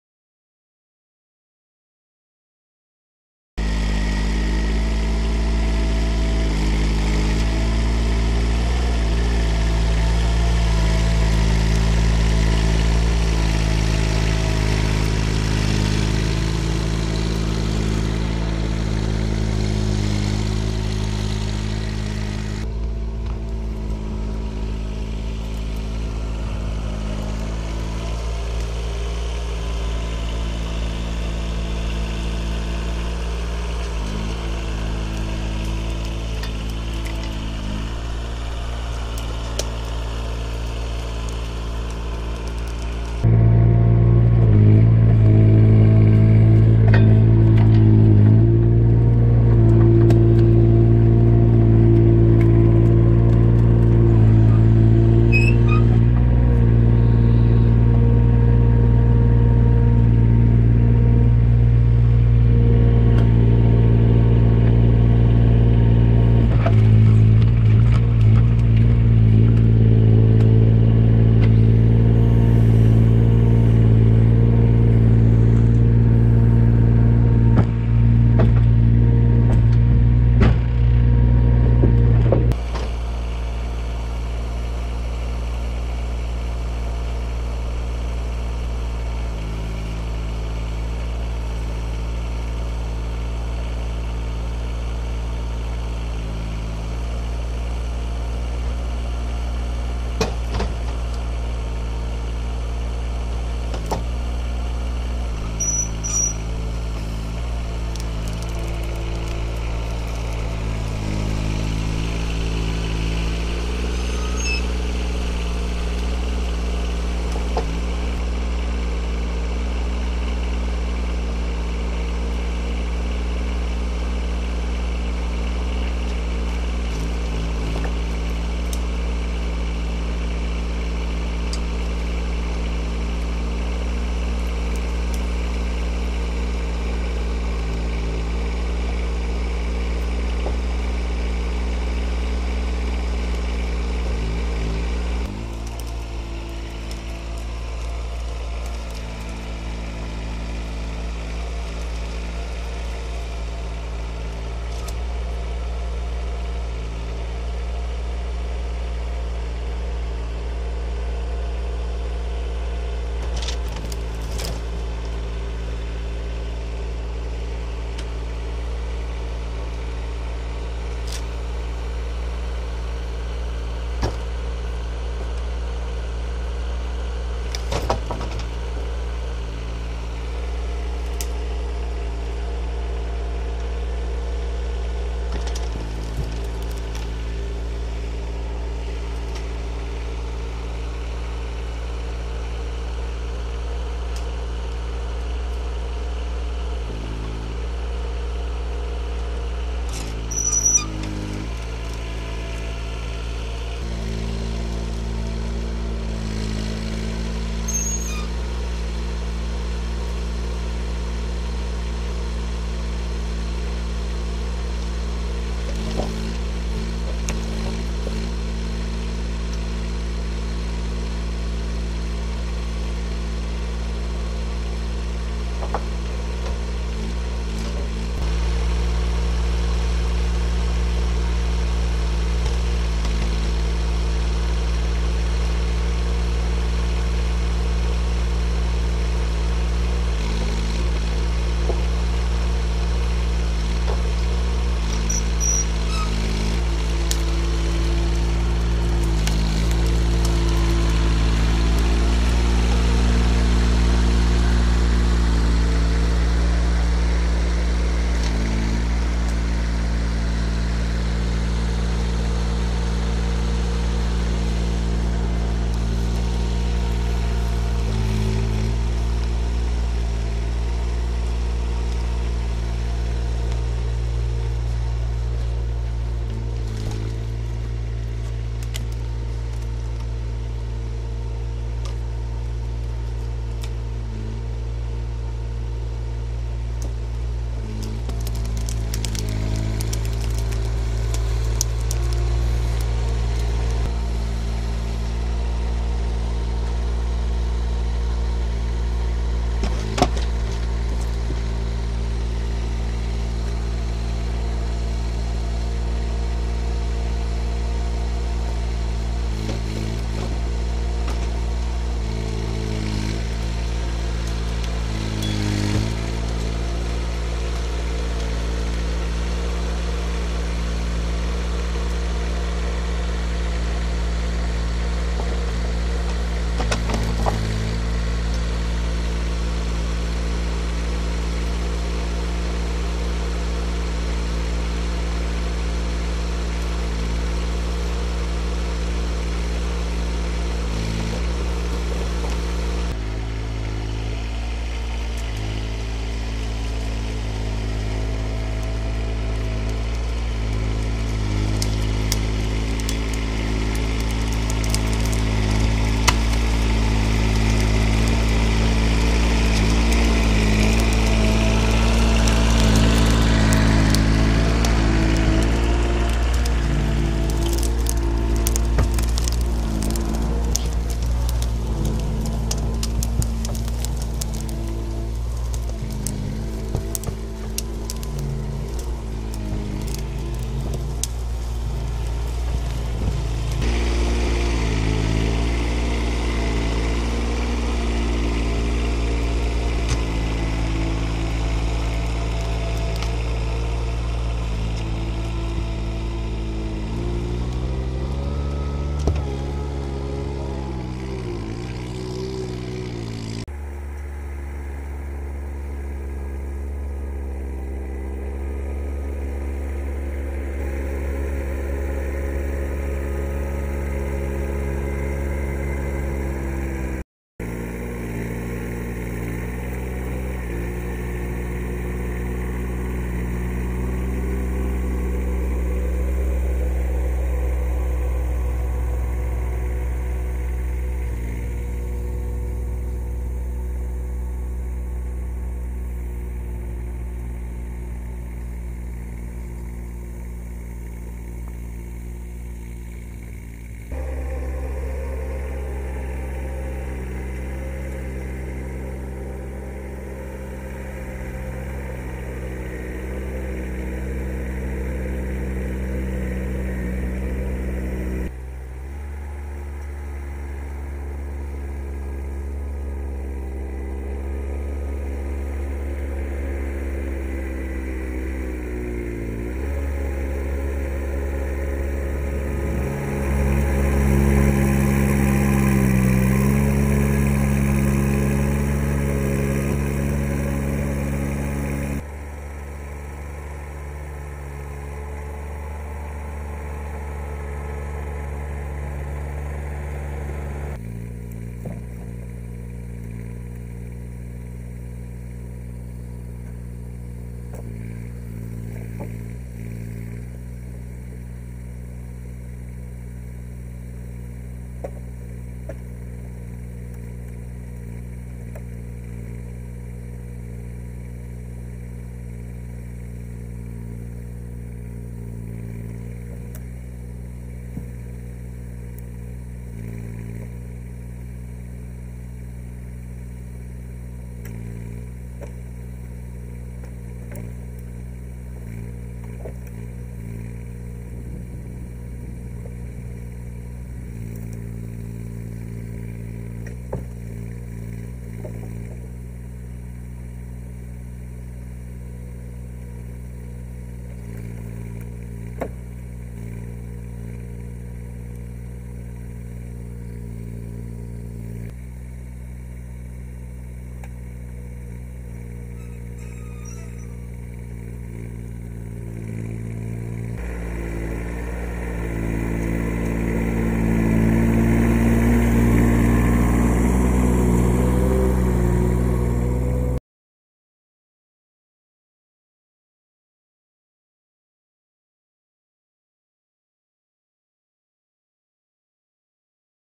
Kranman Bison 10000 skotar förstagallring i 25 cm snö
Kranman Bison 10000 en smidig och lättkörd miniskotare. I filmen kör vi en gallring som är avverkad med vår Skördare 520s. Vi har även krandämpare på kranen som gör den mjukare att köra. Bison har hydrostatik drivning men en körpedal i golvet samt fram och bak knapp.
I filmen är Bison 10000 utrustad med:

Hytt med värme
M36 pro kran
Dubbelkommando
Transportväxel
Hv500 radiostyrd vinsch
2 spak el on/off
Extra ledljus
Bärgnings vinsch fram
Extra förvaring